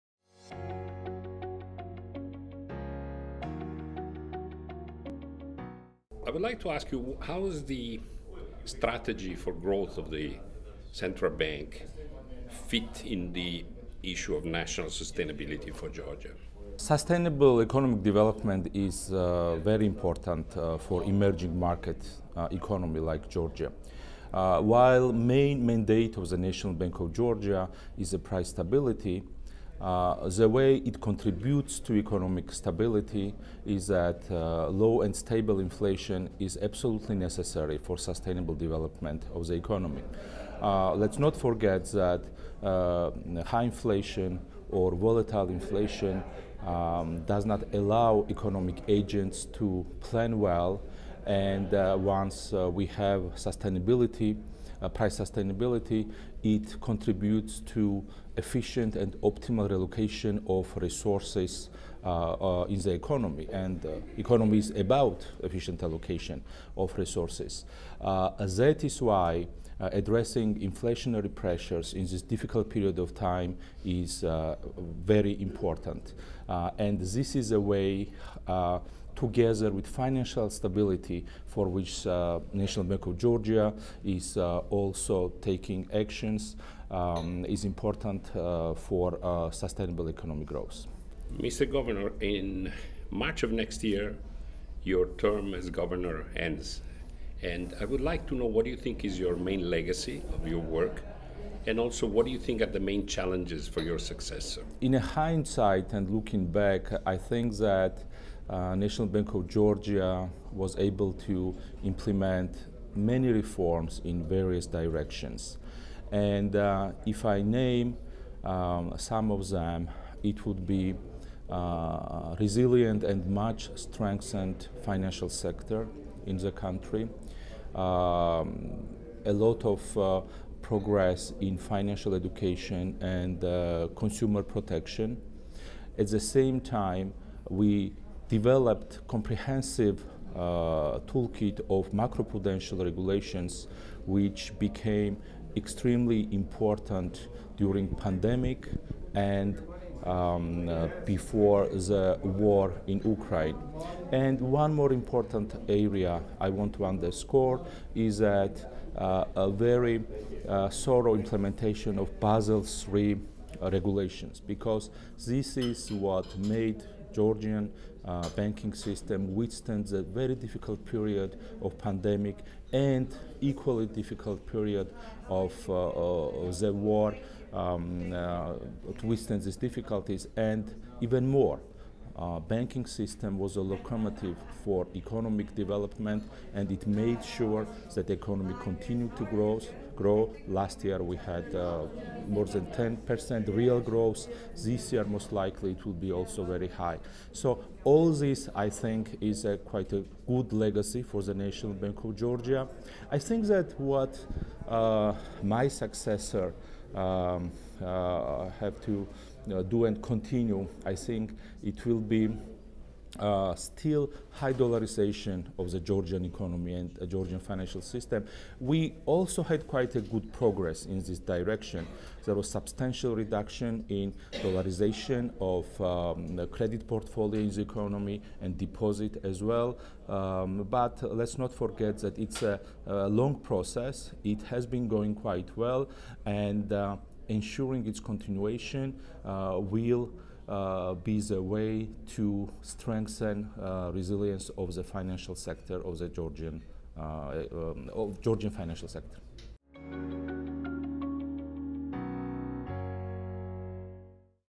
In Conversation with Koba Gvenetadze
Global Finance editor Andrea Fiano interviews Koba Gvenetadze, Central Bank Governor of Georgia during Global Finance's World's Best Bank Awards at the National Press Club in Washington, DC on October 15th.